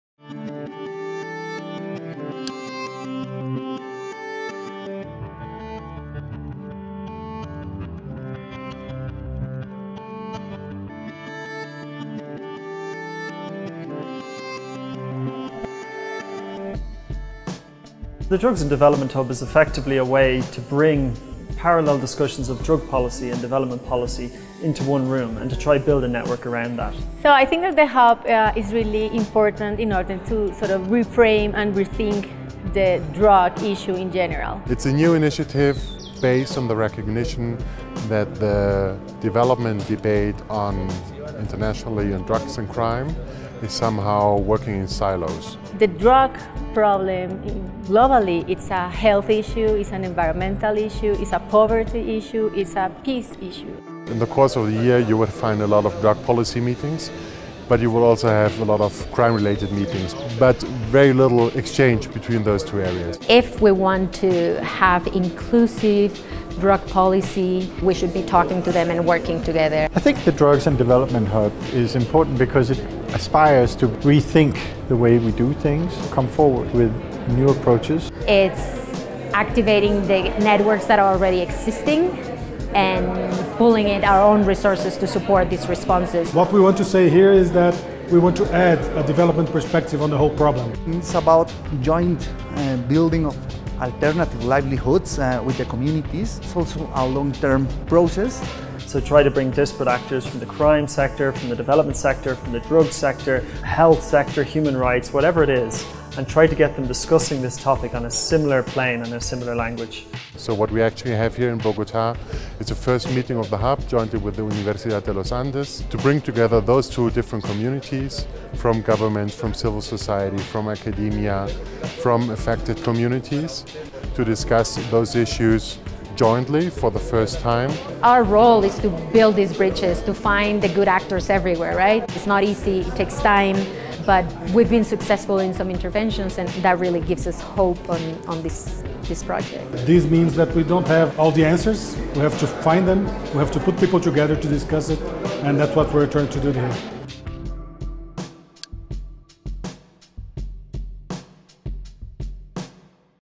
Introducing the Drugs and Development Hub
Recent developments in global drug policy have demonstrated a need to shift the debate and consider the issue from a sustainable development perspective.

The Drugs and Development Hub intends to do just this. An initiative of LSE’s International Drug Policy Unit (IDPU), the GIZ Global Partnership on Drugs Policies and Development (GPDPD) and the Global Initiative on Transnational Organized Crime (GITOC), the Drugs and Development Hub will bring together actors from various fields to help shape new approaches to drugs and development.

In January 2020 we launched the Hub at an event in Colombia hosted by our international partners the Centre for the Study of Security and Drugs (CESED) at the Universidad de Los Andes in Bogota.